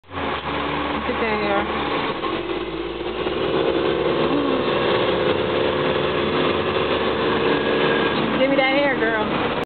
SHE SOLD HER HAIR TO ME I WILL MAKE U A FULL LACE :)
MOST GIRLS HERE HAIR IS THAT LONG HERE BUT THEY R NOT SELLING IT